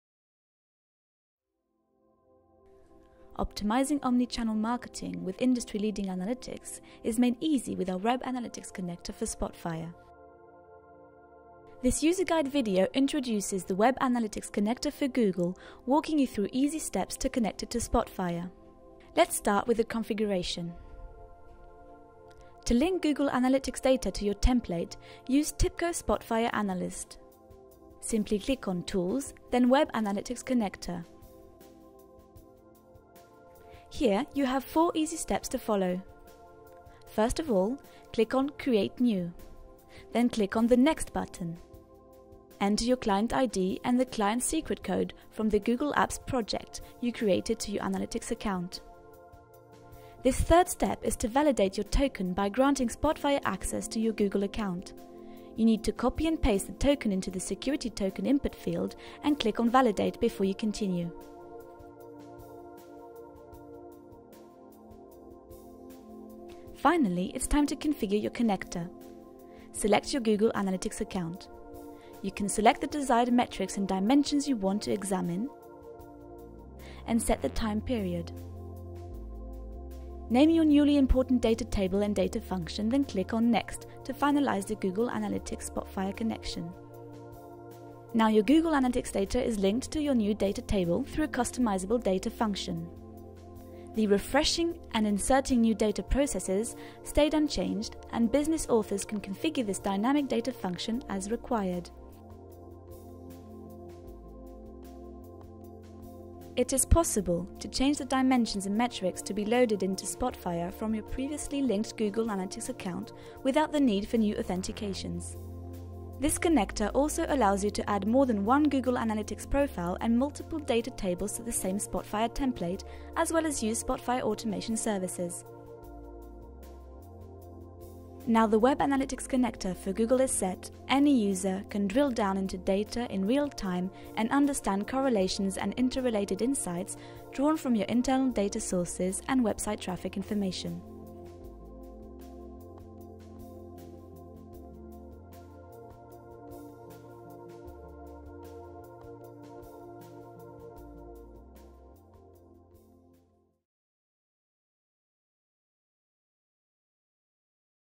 Using Google Analytics data in TIBCO Spotfire®
This user guide video shows you how to connect and use the InfomatiX Web Analytics Connector extension with TIBCO Spotfire®. This way Spotfire users are enabled to use web traffic data in a cutting-edge analytical environment and mash-up with other corporate data.